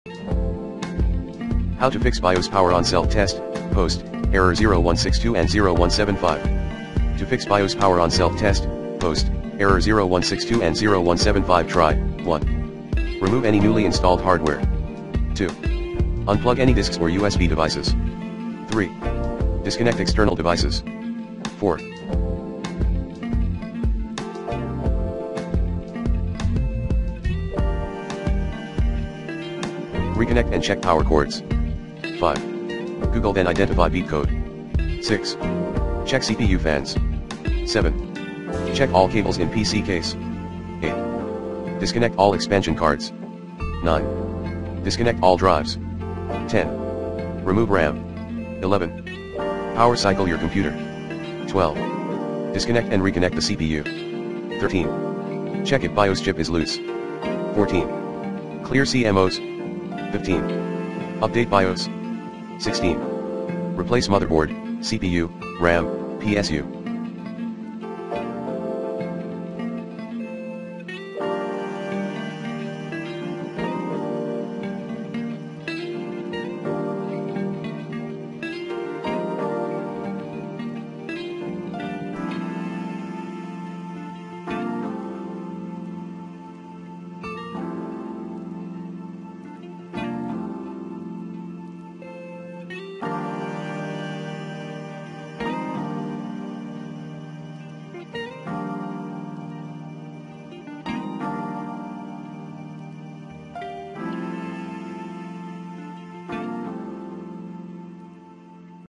Fix BIOS Power-On Self-Test (POST) error 0162 & 0175 on Windows 10/8/7 PC
Here's how to Fix BIOS Power-On Self-Test (POST) error 0162 & 0175 on Windows 10/8/7 PC.